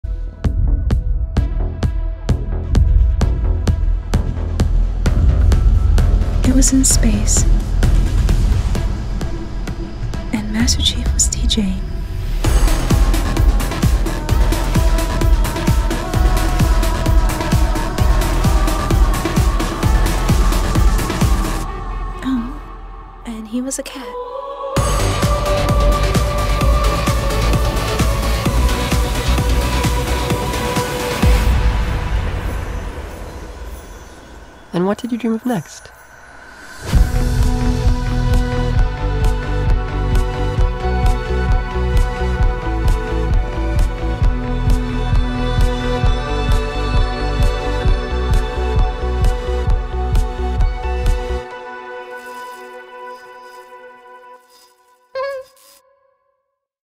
Master Chief DJ Revela su Identidad
Increible, por fin sabemos quien esta detras de ese casco, el Jefe Maestro revela por primera vez su identidad, WOW.